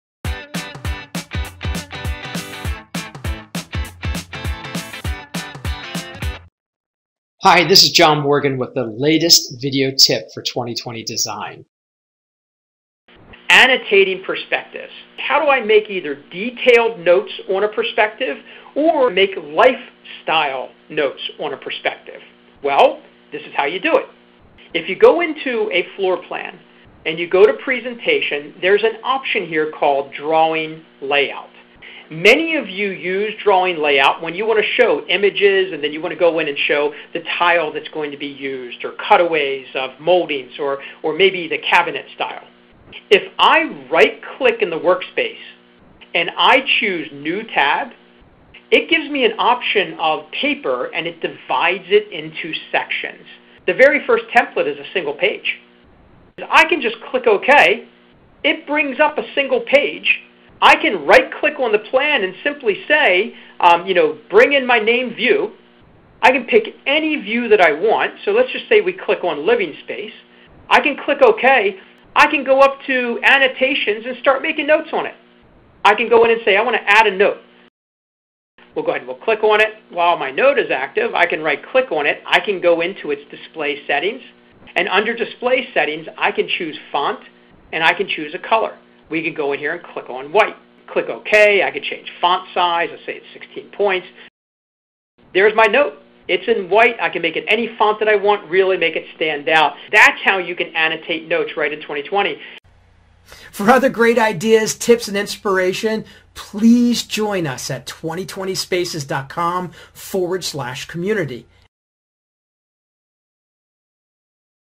2020 Design Tip: Annotating Perspectives
Join John Morgan for this 2020 Design video tip, where you'll learn how to annotate perspectives.